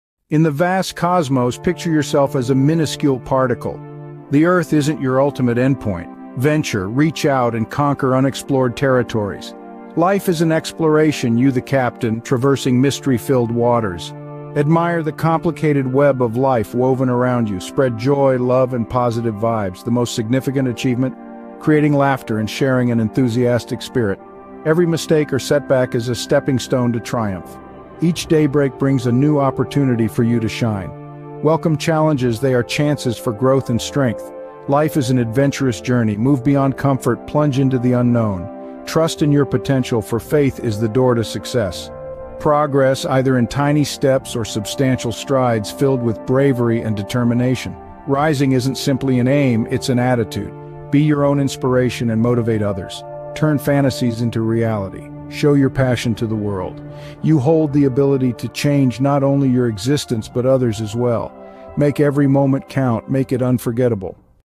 Source Of Life Inspiration | Dynamic Journey
The passage primarily conveys the meaning of exploration, encouragement, and the power of a positive mindset in life. It encourages readers to envision themselves as a small part of the vast universe, urging them to broaden their perspective, reach beyond current limits, and conquer new challenges. Additionally, the passage emphasizes the significance of spreading joy, love, and positivity to both oneself and the surrounding community. It highlights the strength of self-motivation and inspiring others, encouraging the transformation of dreams into reality and the creation of memorable moments in life.